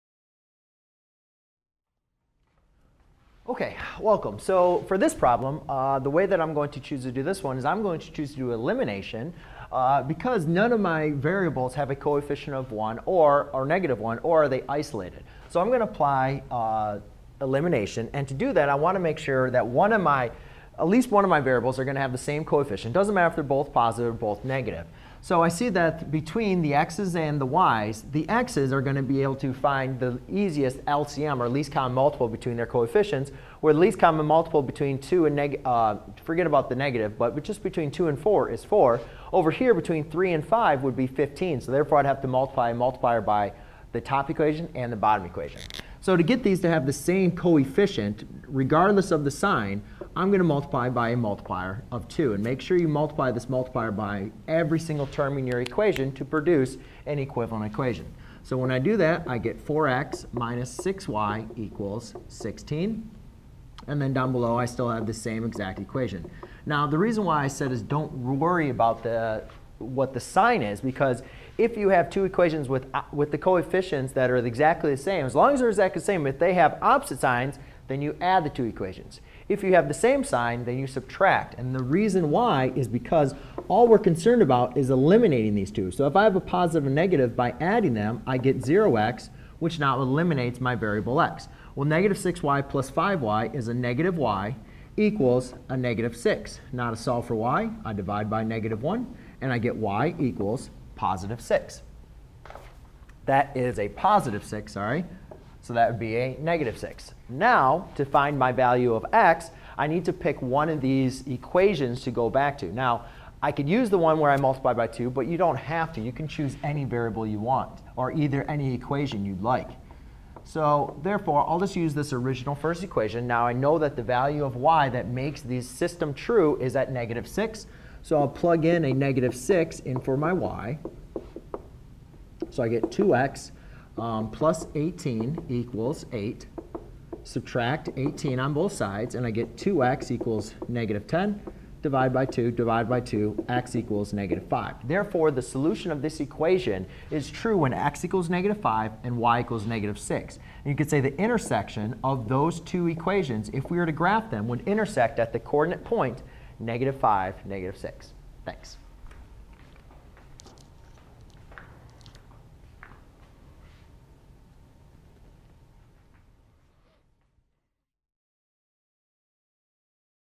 Tutorial - How do we solve a system of linear equations using any method 2x-3y=8, -4x+5y=-10
Learn how to solve a system of equations by using any method such as graphing, elimination and substitution. 2x-3y=8, -4x+5y=-10